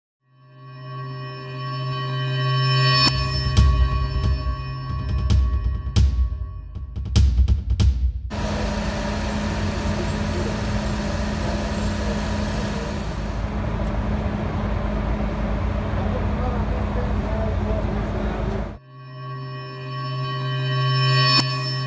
Carambola entre unidades del transporte público en Calzada Ignacio Zaragoza en Puebla
Cuerpos de emergencia atendieron una carambola de tres unidades del transporte público de Puebla, sobre la Calzada Ignacio Zaragoza y Diagonal Defensores de la República.